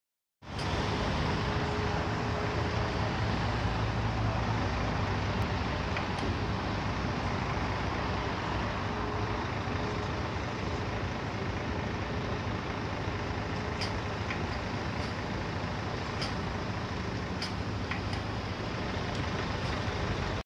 The Weather Over York.
This was the weather over York North Yorkshire on Friday 9 October 2015 when I was on my way home from work.